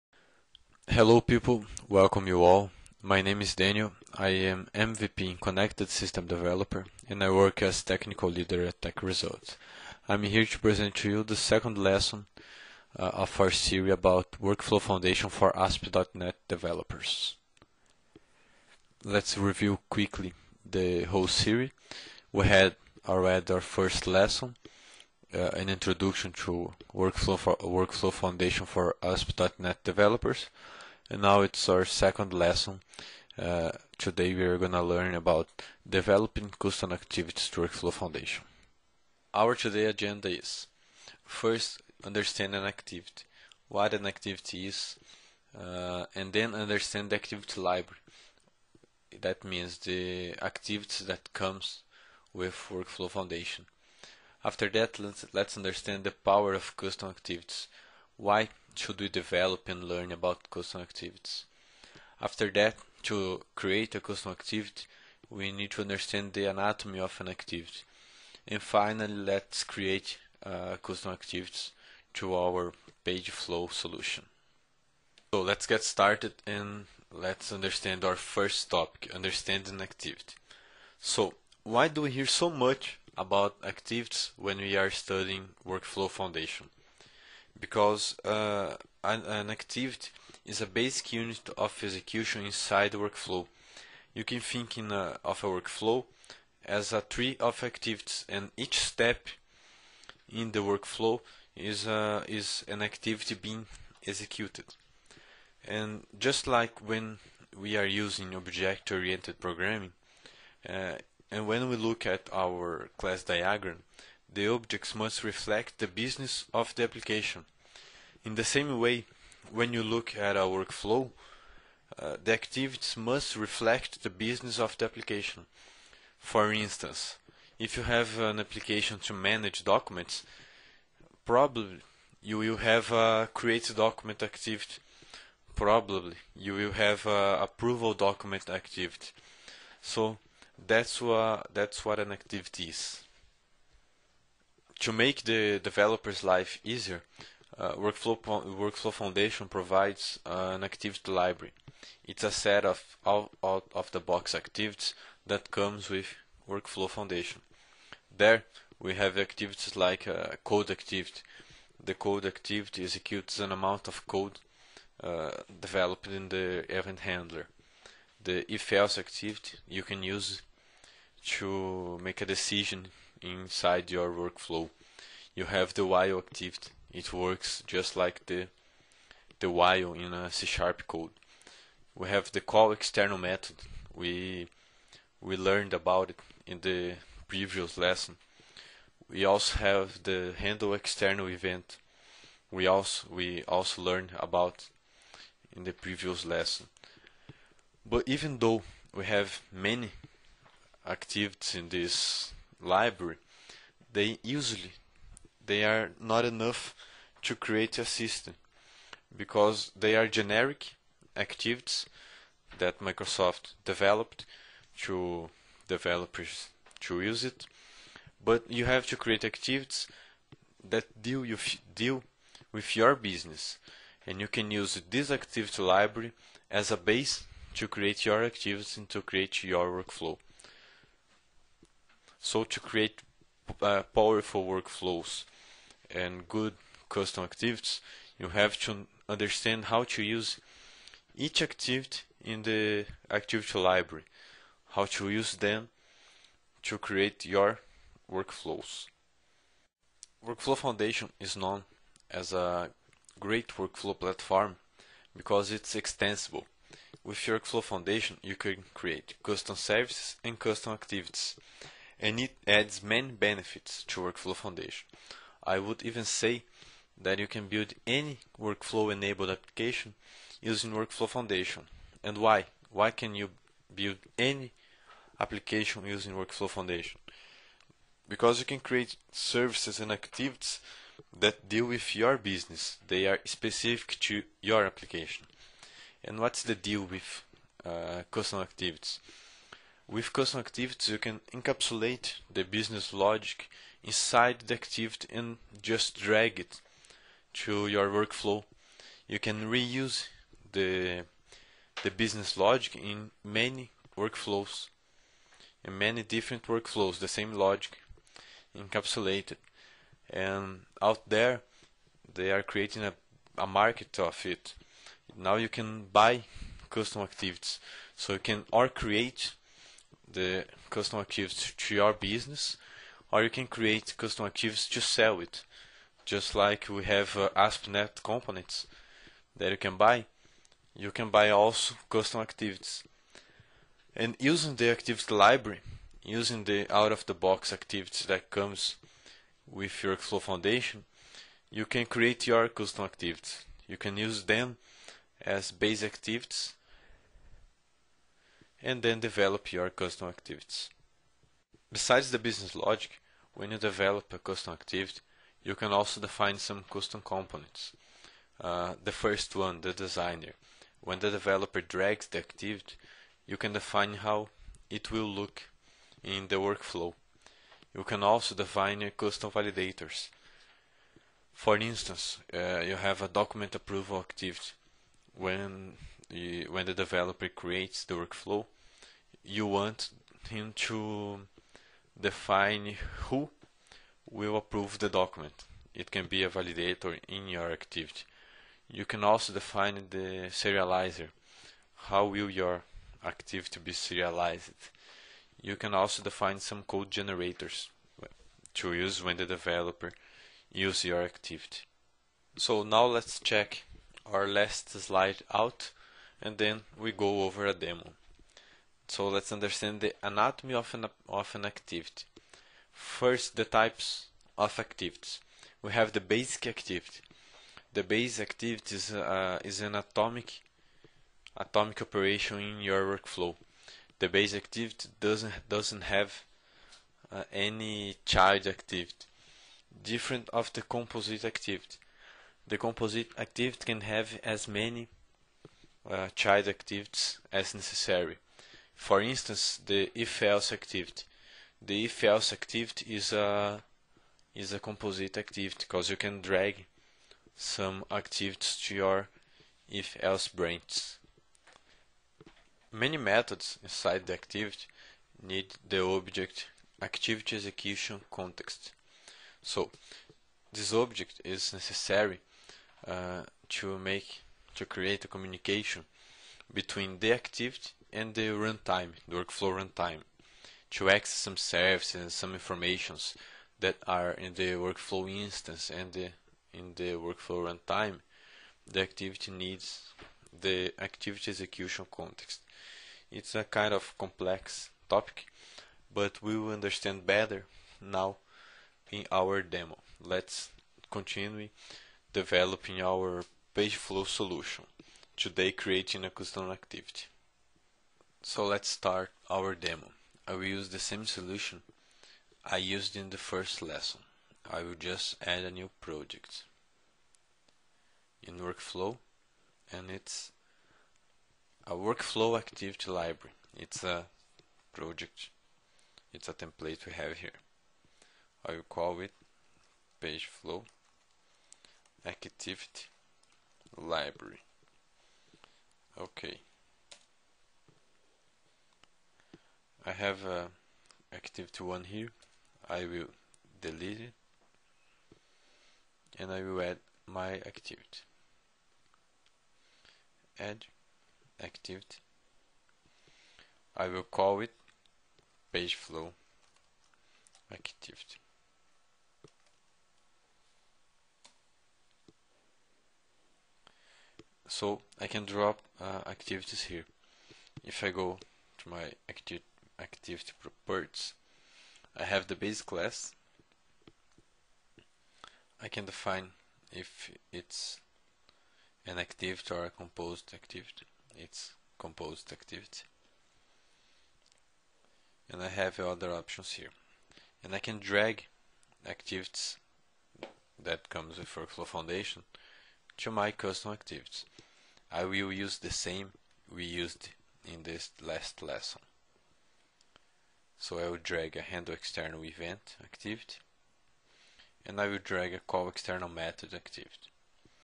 E learning Workflow Foundation for ASP NET Developers Lesson 2 (part 1)
Windows Workflow Foundation is part of the Microsoft .NET Framework 3.5. Is this series of lessons we will understand how to develop a Page-flow solution using ASP.NET and Windows Workflow Foundation.In this lesson we will learn about activities. We will understand how to create custom activities that deal with the business of the application and why custom activities make workflow foundation be a powerful workflow platform. To create custom activities we will learn about the anatomy of an activity, the difference between a Basic Activity and a Composite Activity. To create good workflows and good custom activities we will learn about the set of out-of-the-box activities that comes with Workflow Foundation.In our demo we will continue developing our Page-flow solution. In this lesson we will create a PageFlowActivity with a custom design and a Workflow to use this activity.

Technologies Used: Windows Workflow Foundation, .NET Framework 3.5, Visual Studio 2008.